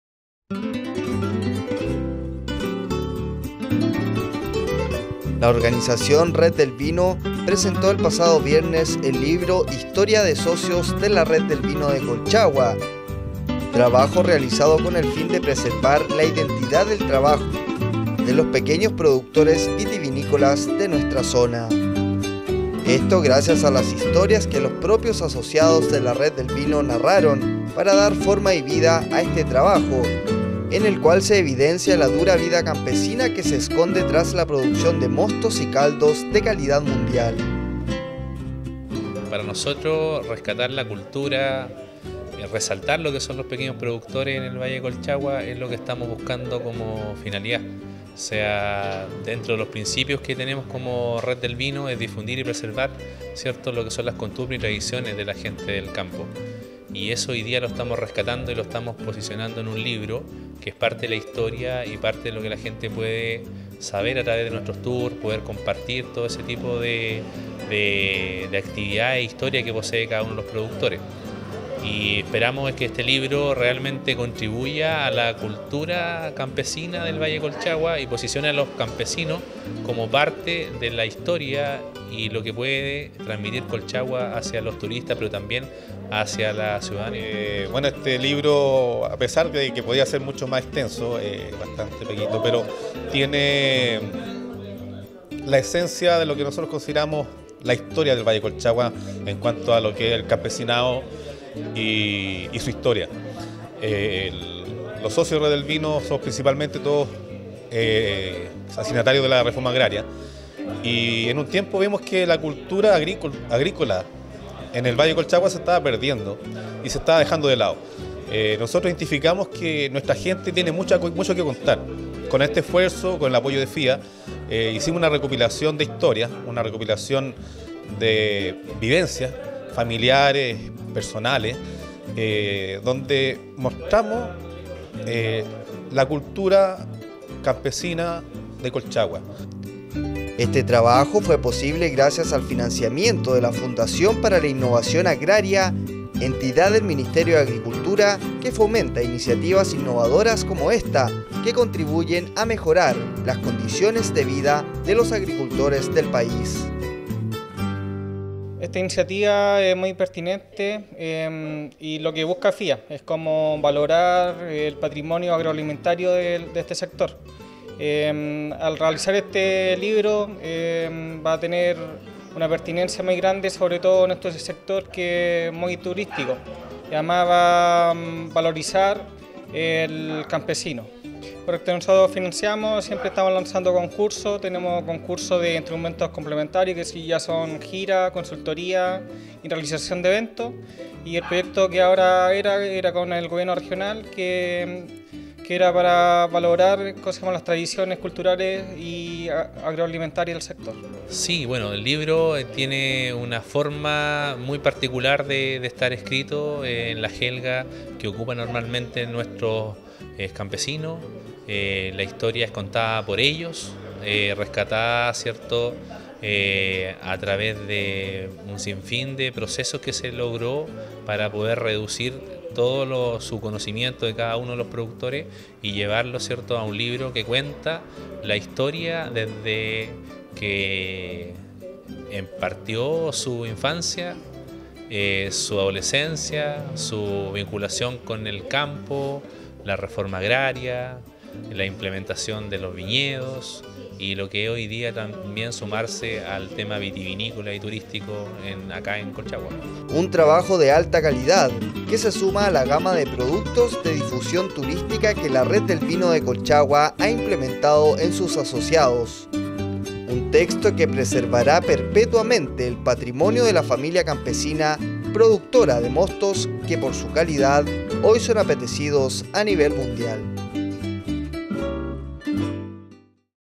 Lanzamiento Libro Red del Vino Colchagua
La organización Red el Vino de Colchagua presentó el libro “Historias Socios de la Red del Vino de Colchagua“, trabajo realizado con el fin de preservar la identidad del trabajo de los pequeños productores vitivinícolas de nuestra zona.

Esto, gracias a las historias que los propios asociados a la Red el Vino narraron, para dar forma y vida a este trabajo, en el cual se evidencia la dura vida campesina que se esconde tras la producción de mostos y caldos de calidad mundial.